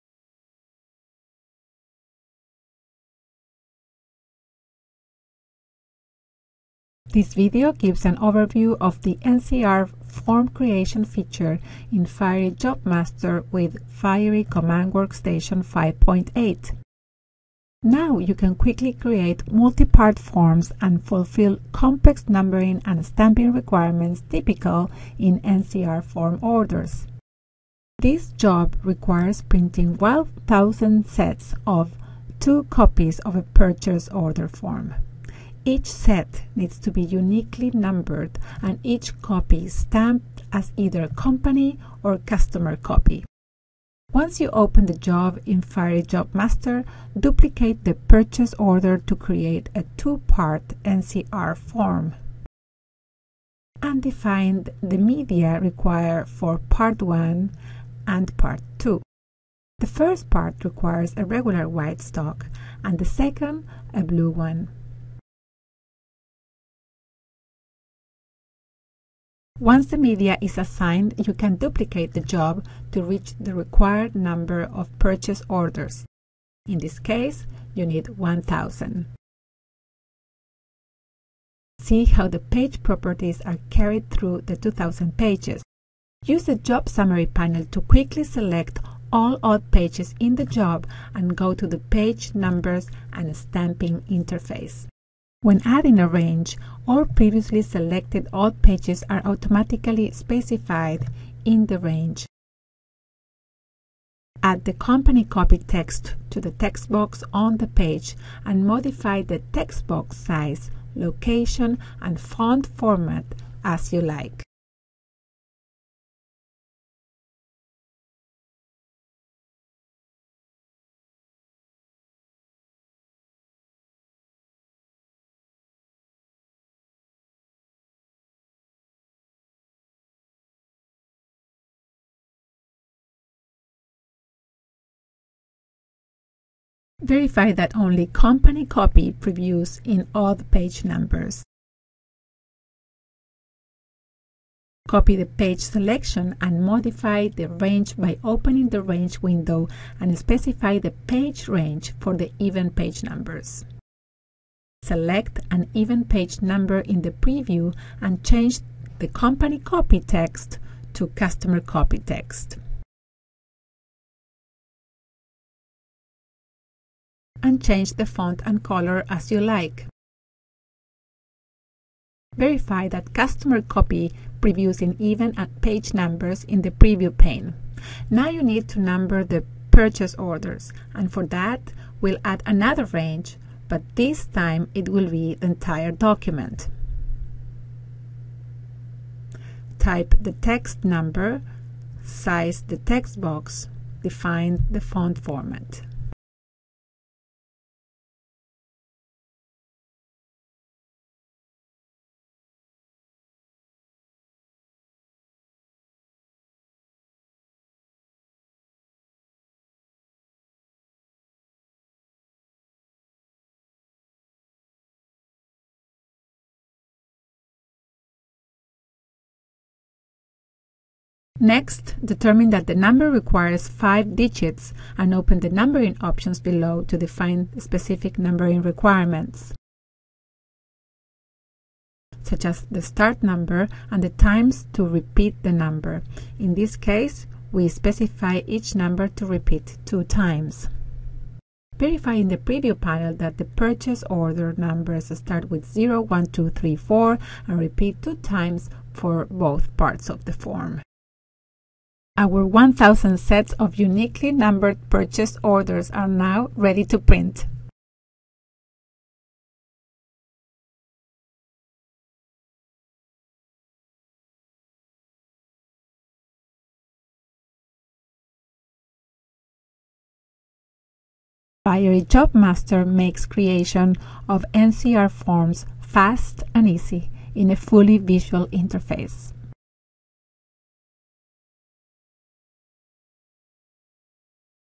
Fiery JobMaster: NCR form creation
Quickly create multi-part forms and fulfill complex numbering and stamping requirements typical in NCR form orders.